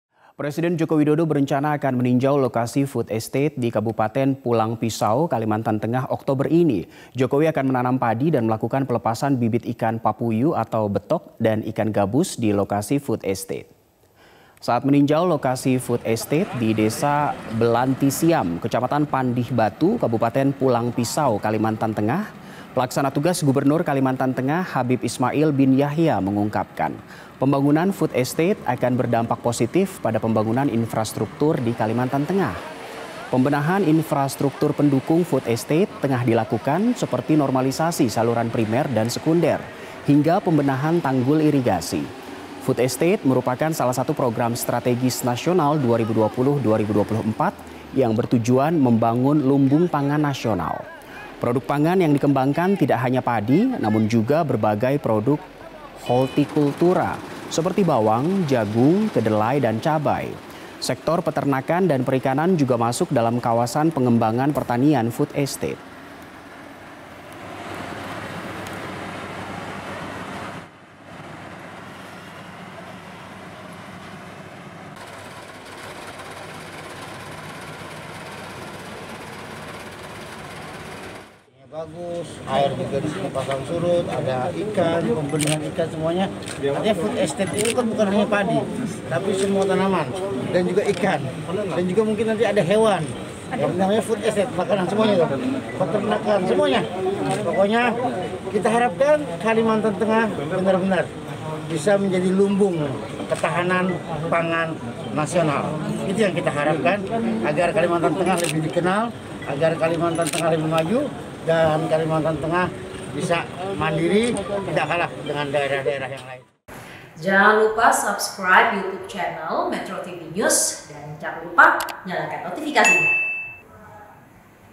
Jokowi akan Tinjau Food Estate di Kalteng
Presiden Joko Widodo berencana meninjau lokasi food estate di Kabupaten Pulang Pisau, Kalimantan Tengah. Ia akan menanam padi dan melakukan pelepasan bibit ikan papuyu atau betok dan ikan gabus di lokasi itu.